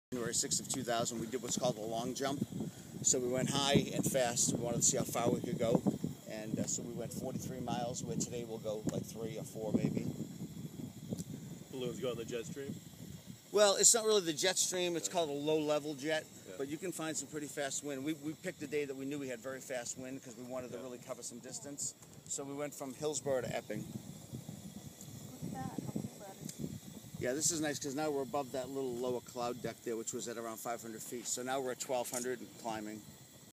Flying really high in a hot air balloon
In this video we are ascending to 1300 feet in a hot air balloon in Salem New Hampshire. Thanks Tony for a awesome experience.